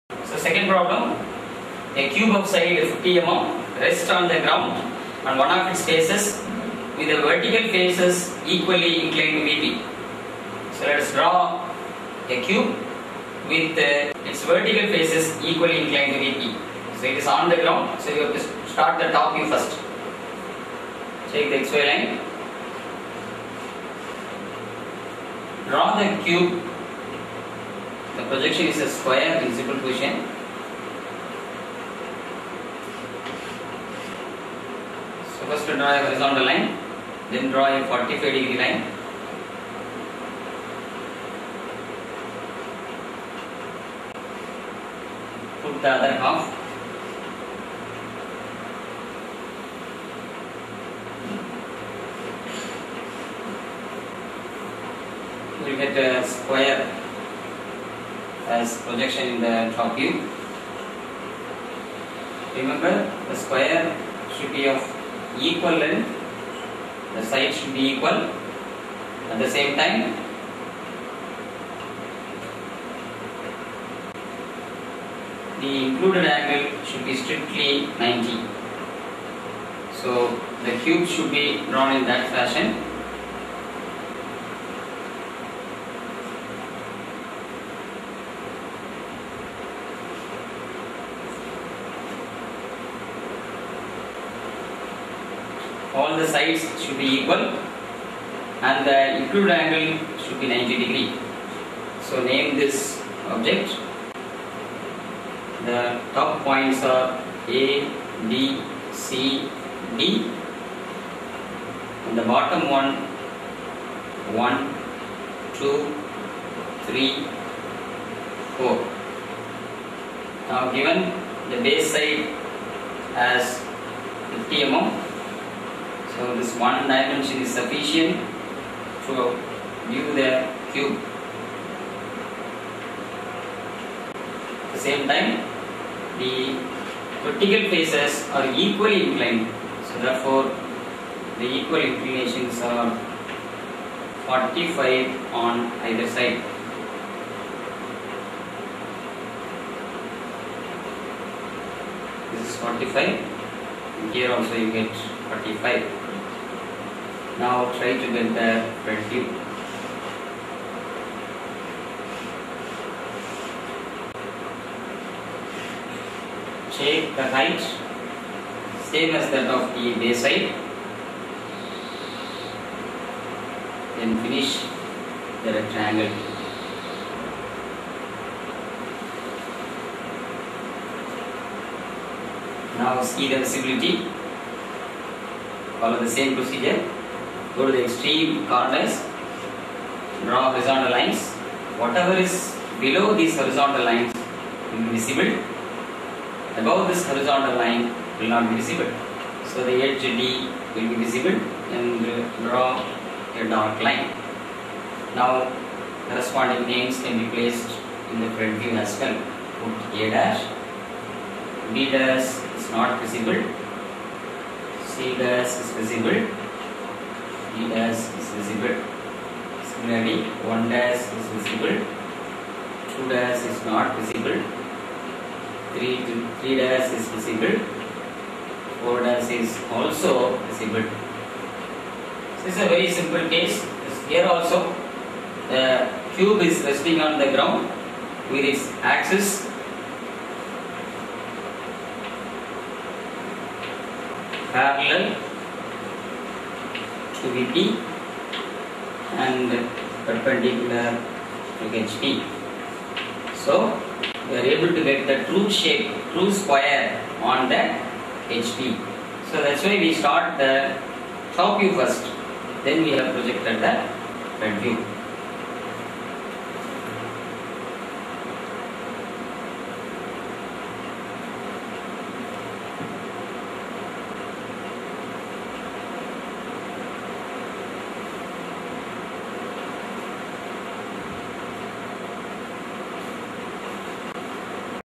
Engineering Graphics MADE EASY by Prof.Dr.R.Edison -Projections of Solids - Problem 02
As per Anna University syllabus for first year engineering students